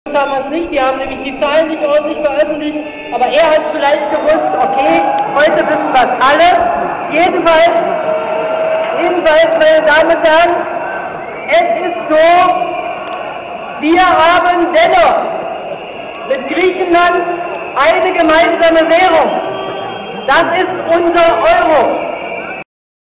Wahlkampfrede Merkel Wuppertal zu Griechenland
Die Griechenlandkrise als Thema bei der Wahlkampfveranstaltung der CDU in Wuppertal anläßlich der Landtagswahl NRW. Rede von Angela Merkel.